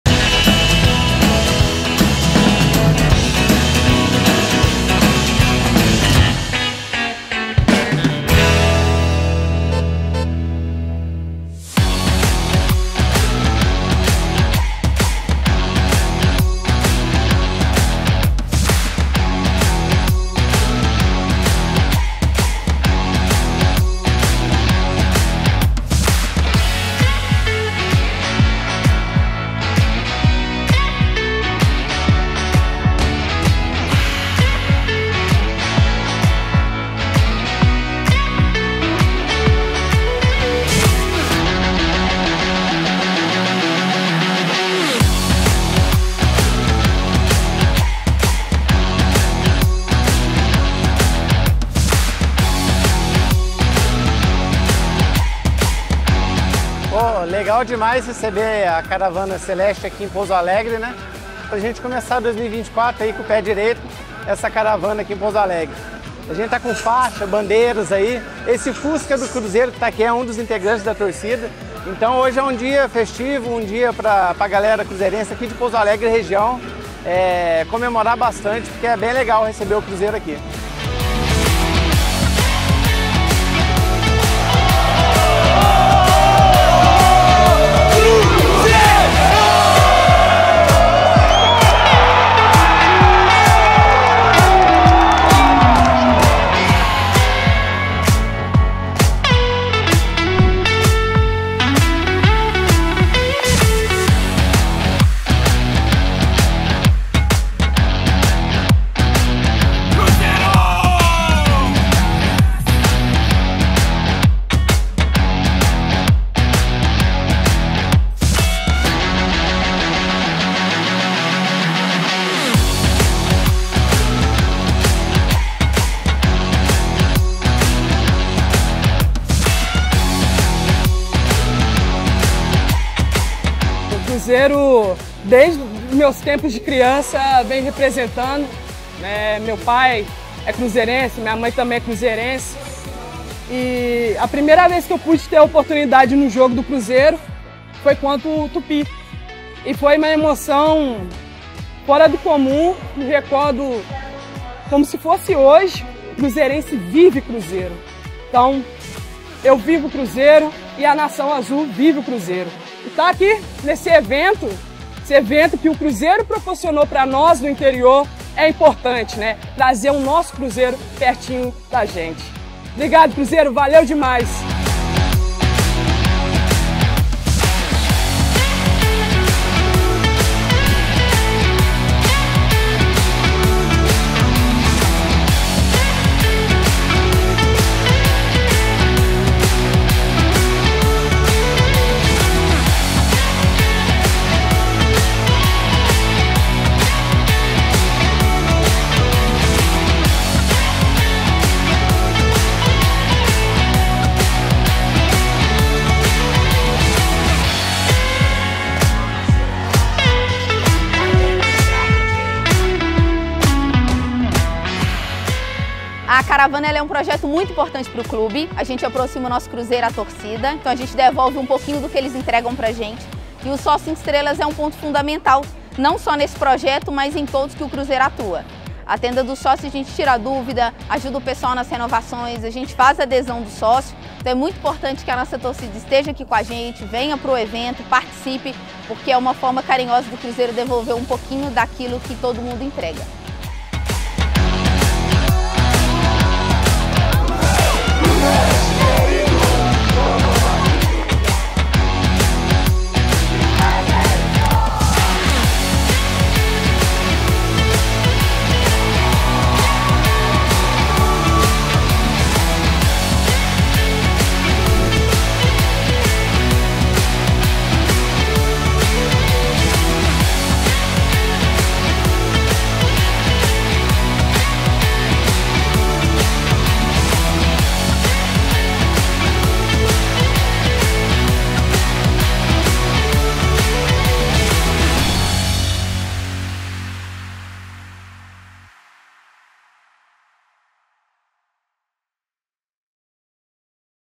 🚍 Confira a retrospectiva da Caravana do Cruzeiro pelo interior de Minas Gerais em 2024!
A CaravanaDoCruzeiro, sucesso de público no interior de Minas Gerais, percorreu Pouso Alegre, Juiz de Fora, Betim e Varginha no primeiro semestre de 2024.

Em parceria com o SESC Mesa Brasil, arrecadamos 4.413 kg de alimentos para doação nas quatro cidades. Vem ver como foi esse evento de sucesso!